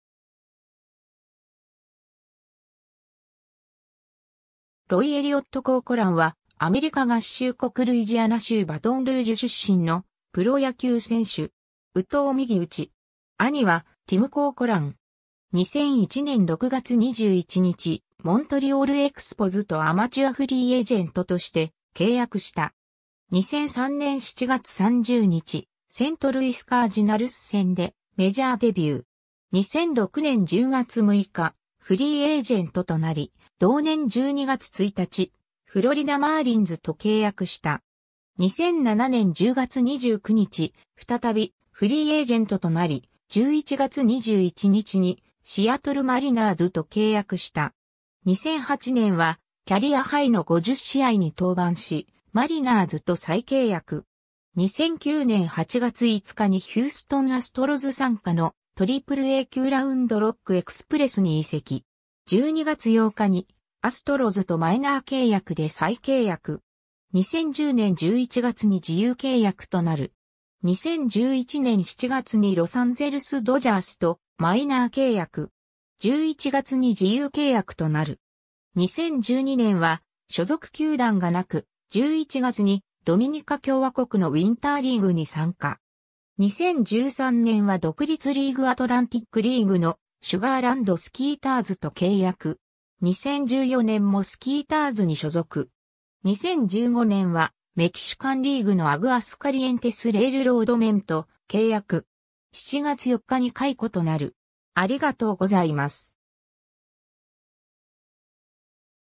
ロイ・コーコラン
アグアスカリエンテス・レイルロードメンの選手
ロイ・エリオット・コーコラン（Roy Elliot Corcoran , 1980年5月11日 - ）は、アメリカ合衆国ルイジアナ州バトンルージュ出身のプロ野球選手（投手）。
右投右打。
 兄はティム・コーコラン。
 2001年6月21日、モントリオール・エクスポズとアマチュア・フリーエージェントとして契約した。
 2003年7月30日、セントルイス・カージナルス戦でメジャーデビュー。
 2006年10月6日、フリーエージェントとなり同年12月1日、フロリダ・マーリンズと契約した。
 2007年10月29日、再びフリーエージェントとなり、11月21日にシアトル・マリナーズと契約した。
 2008年はキャリアハイの50試合に登板し、マリナーズと再契約。
 2009年8月5日にヒューストン・アストロズ傘下のAAA級ラウンドロック・エクスプレスに移籍。
12月8日にアストロズとマイナー契約で再契約。
 2010年11月に自由契約となる。
 2011年7月にロサンゼルス・ドジャースとマイナー契約。
11月に自由契約となる。
 2012年は所属球団がなく、11月にドミニカ共和国のウィンターリーグに参加。
 2013年は独立リーグ・アトランティックリーグのシュガーランド・スキーターズと契約。
 2014年もスキーターズに所属。
 2015年はメキシカンリーグのアグアスカリエンテス・レイルロードメンと契約。
7月4日に解雇となる。